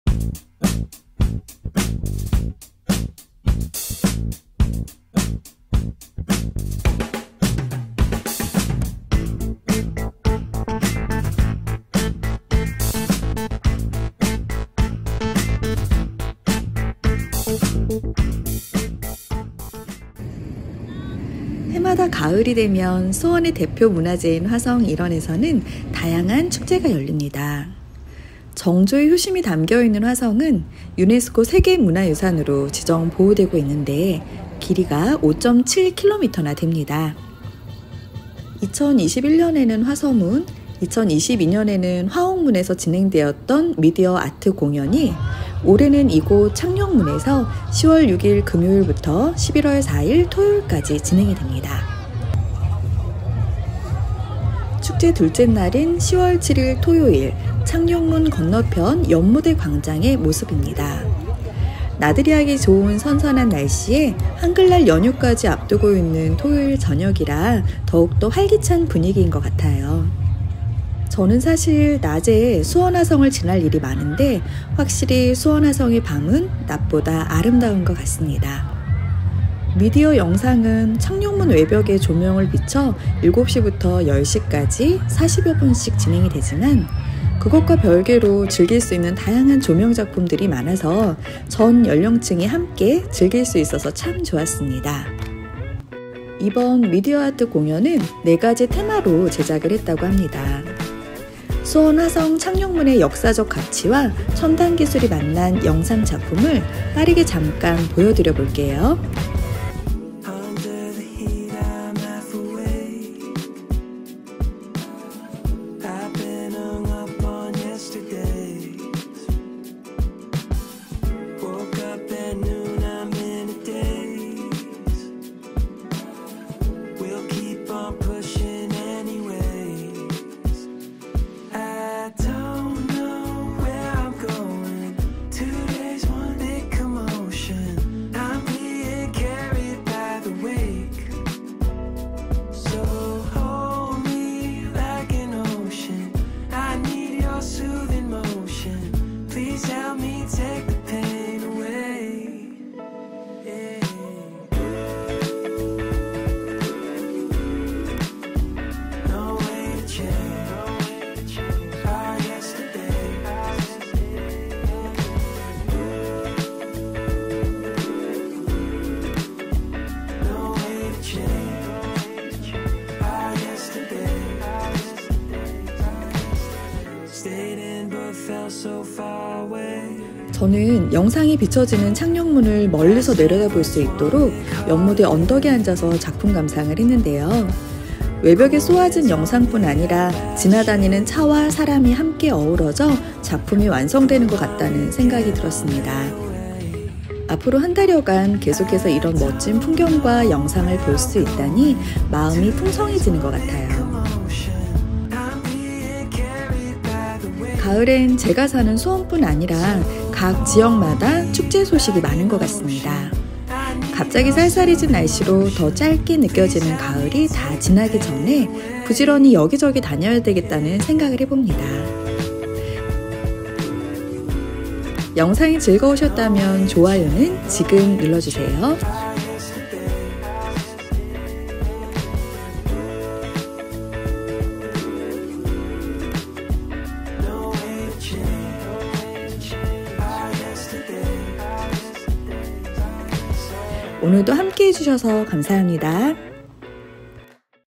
2023년 주말나들이 야간데이트코스추천 가을 수원화성 미디어아트 Media Art Suwon Hwaseong
📌 수원화성미디어아트

- 일정 : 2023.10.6.(금)~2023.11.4.(토)[30일간] / 19:00~22:00
- 장소 : 창룡문 일원

1:50 2023수원화성미디어아트 미리보기😁